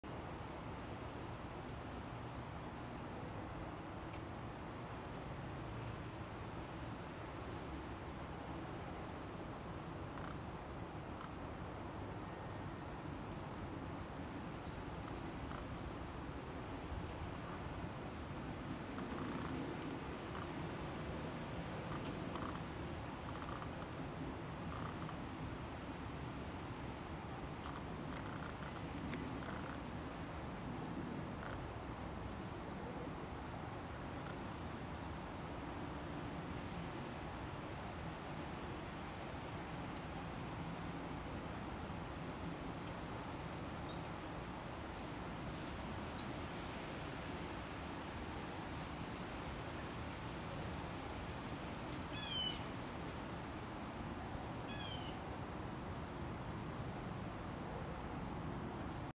Sleeping Squirrel
Regular morning visitor - stopped in for a nap.

Canon EOS R5
AngelBird AV Pro 512GB CFExpress Type B Card